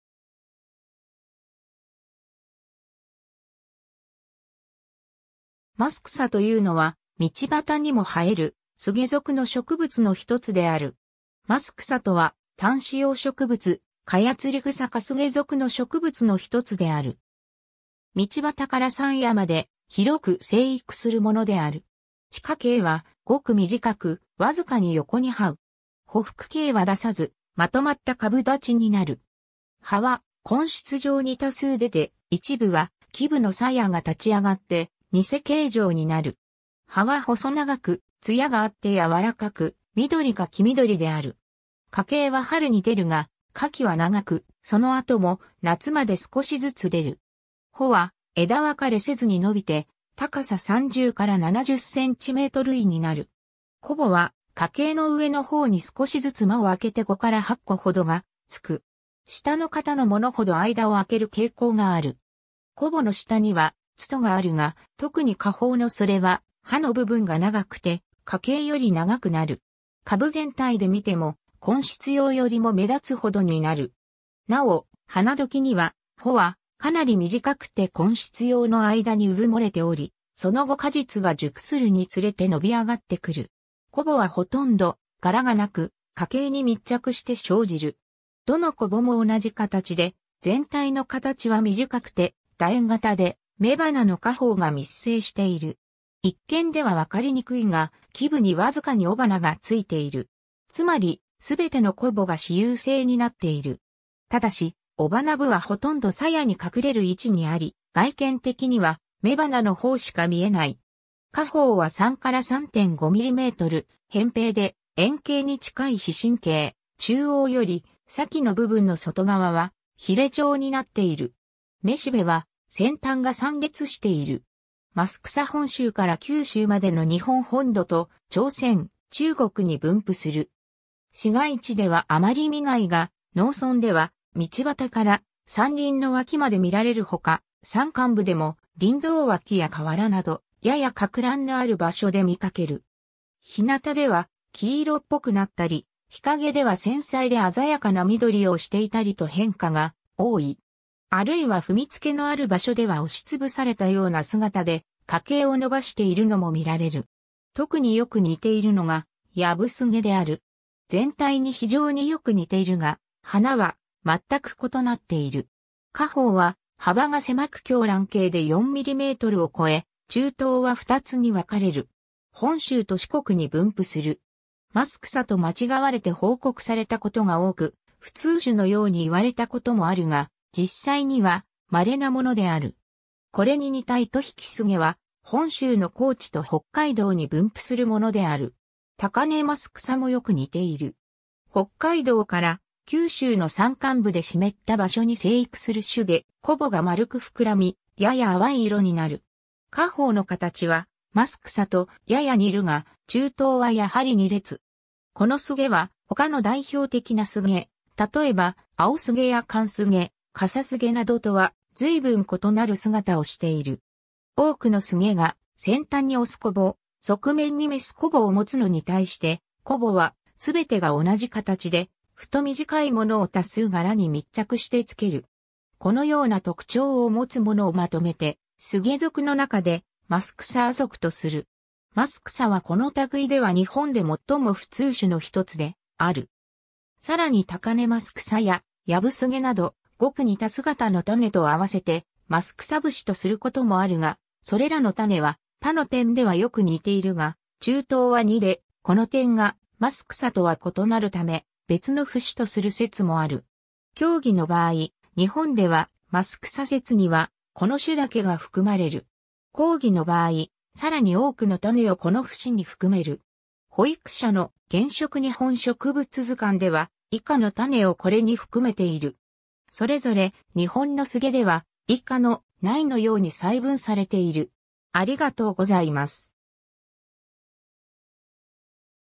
マスクサ
マスクサというのは、道端にも生えるスゲ属の植物のひとつである。
 マスクサ(マスクサスゲとも Carex gibba Wahlenb.)とは、単子葉植物カヤツリグサ科スゲ属の植物のひとつである。
道端から山野まで、広く生育するものである。
 地下茎はごく短く、わずかに横に這う。
匍匐茎は出さず、まとまった株立ちになる。
葉は根出状に多数出て、一部は基部の鞘が立ち上がって偽茎状になる。
葉は細長く、つやがあって柔らかく、緑か黄緑である。
 花茎は春に出るが、花期は長く、その後も夏まで少しずつ出る。
穂は枝分かれせずに伸びて、高さ30-70cm位になる。
小穂は花茎の上の方に少しずつ間を開けて5-8個ほどがつく。
下の方のものほど間を開ける傾向がある。
小穂の下には苞があるが、特に下方のそれは葉の部分が長くて、花茎より長くなる。
株全体で見ても、根出葉よりも目立つほどになる。
なお、花時には穂はかなり短くて根出葉の間に埋もれており、その後果実が熟するにつれて伸び上がってくる。
 小穂はほとんど柄がなく、花茎に密着して生じる。
どの小穂も同じ形で、全体の形は短くて楕円形で、雌花の果胞が密生している。
一見では分かりにくいが、基部にわずかに雄花が着いている。
つまり、すべての小穂が雌雄性になっている。
ただし、雄花部はほとんど鞘に隠れる位置にあり、外見的には雌花の方しか見えない。
 果胞は3-3.5mm、偏平で円形に近い披針形、中央より先の部分の外側は鰭状になっている。
雌しべは先端が三裂している。
 マスクサ（やや日影の株） 本州から九州までの日本本土と、朝鮮、中国に分布する。
 市街地ではあまり見ないが、農村では道端から山林の脇まで見られるほか、山間部でも林道脇や河原など、やや撹乱のある場所で見かける。
日なたでは黄色っぽくなったり、日陰では繊細で鮮やかな緑をしていたりと変化が多い。
あるいは踏み付けのある場所では押し潰されたような姿で花茎を伸ばしているのも見られる。
 特によく似ているのがヤブスゲ(C. rochebrunii Franch. et Savat.)である。
全体に非常によく似ているが、花は全く異なっている。
果胞は幅が狭く狭卵形で4mmを越え、柱頭は2つに分かれる。
本州と四国に分布する。
マスクサと間違われて報告されたことが多く、普通種のように言われたこともあるが、実際にはまれなものである。
これに似たイトヒキスゲ(C. remotiuscula Wshlenb.)は、本州の高地と北海道に分布するものである。
 タカネマスクサ(C. planata Franch. et Savat.)もよく似ている。
北海道から九州の山間部で湿った場所に生育する種で、小穂が丸く膨らみ、やや淡い色になる。
果胞の形はマスクサとやや似るが、柱頭はやはり2裂。
 このスゲは、他の代表的なスゲ、例えばアオスゲやカンスゲ、カサスゲなどとは随分異なる姿をしている。
多くのスゲが先端に雄小穂、側面に雌小穂を持つのに対して、小穂はすべてが同じ形で、太短いものを多数柄に密着してつける。
このような特徴を持つものをまとめてスゲ属の中でマスクサ亜属とする。
マスクサはこの類では日本で最も普通種のひとつである。
 さらにタカネマスクサやヤブスゲなど、ごく似た姿の種と合わせてマスクサ節とすることもあるが、それらの種は、他の点ではよく似ているが、柱頭は2で、この点がマスクサとは異なるため、別の節とする説もある。
狭義の場合、日本ではマスクサ節にはこの種だけが含まれる。
 広義の場合、さらに多くの種をこの節に含める。
保育社の『原色日本植物図鑑』では以下の種をこれに含めている。
それぞれ『日本のスゲ』では以下の（）内のように細分されている。